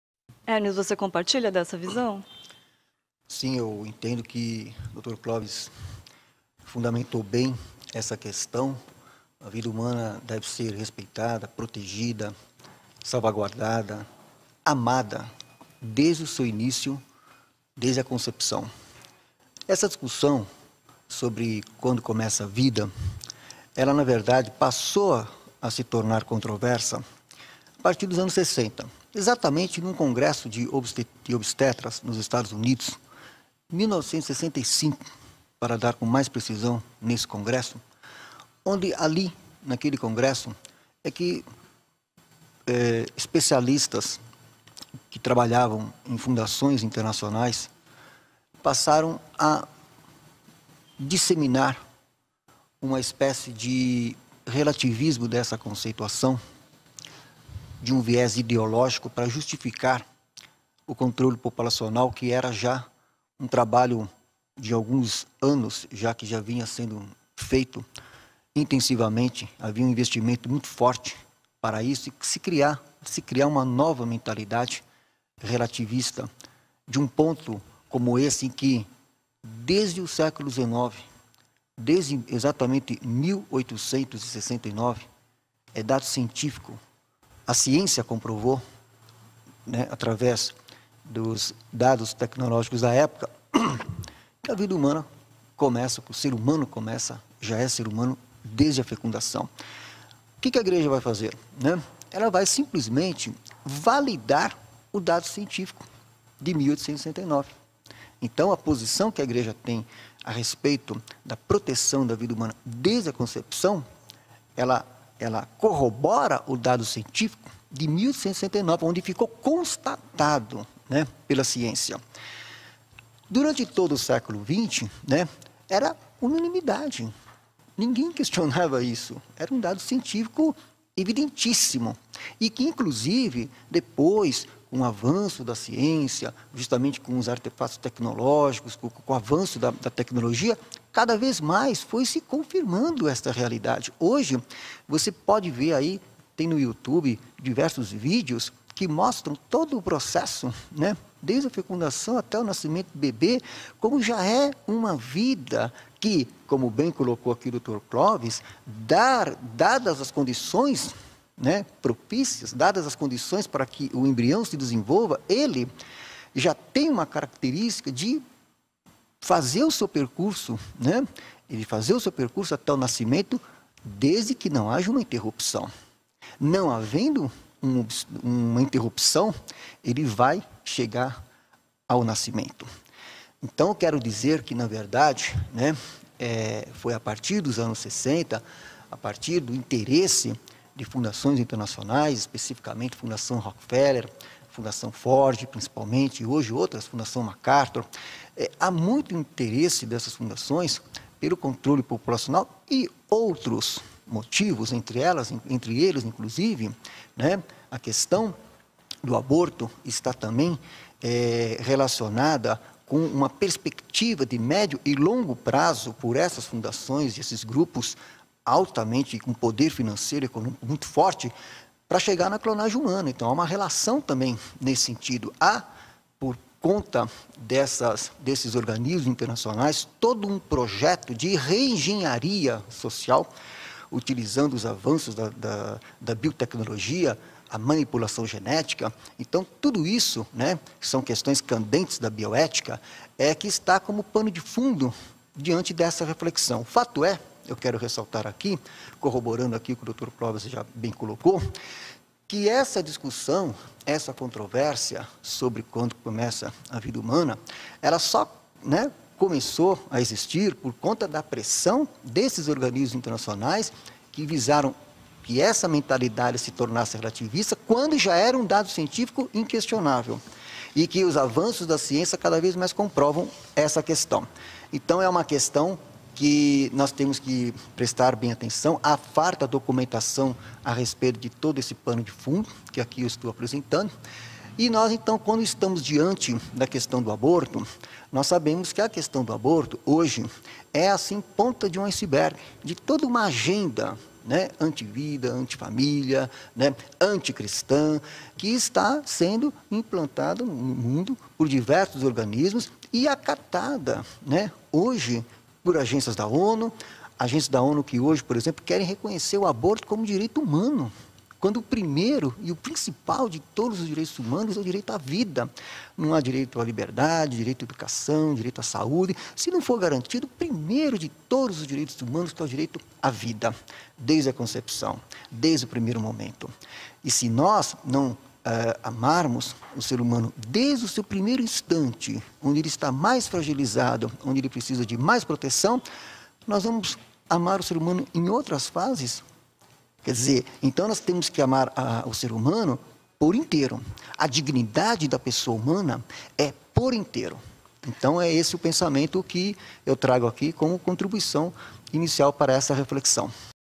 "A VIDA HUMANA DEVE SER PROTEGIDA E AMADA, DESDE A CONCEPÇÃO"
Em debate ocorrido na Câmara Municipal de Guarulhos, em fevereiro de 2015, o Prof. Hermes Rodrigues Nery, Presidente da Associação Nacional Pró-Vida e Pró-Família e especialista em Bioética, explica quando e porque se relativizou o que era dado científico inquestionável: o início, da vida humana com a concepção. Para ele, desde o início, a vida humana deve ser amada e protegida, pois a vida humana vale por inteiro.